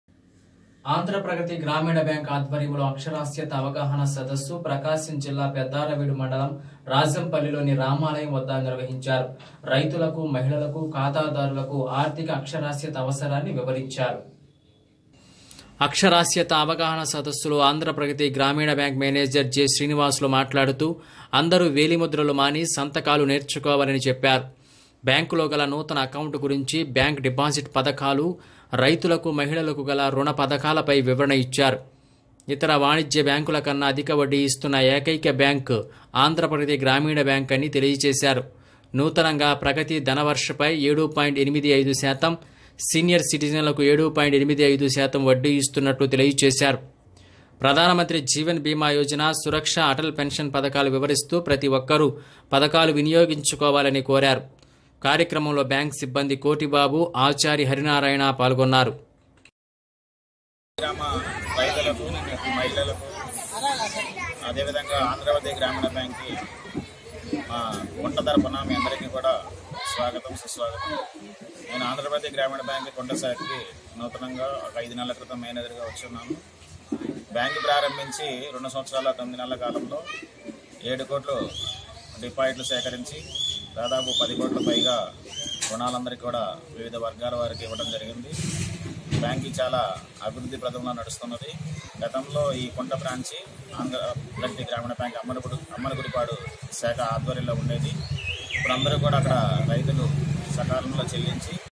Andhra Pragathi Grameena Bank Participates in aksharasyatha avagahana sadassu||Peddaravedu|Dn5
ఆంధ్ర ప్రగతి గ్రామీణ బాంక్ ఆధ్వర్యంలో అక్షరాస్యత అవగాహన సదస్సు ప్రకాశం జిల్లా పెద్దారవీడు మండలం రాజంపల్లిలోని రామాలయం వద్ద నిర్వహించారు. రైతులకు, మహిళలకు, ఖాతాదారులకు ఆర్థిక అక్షరాస్యత అవసరాన్ని వివరించారు.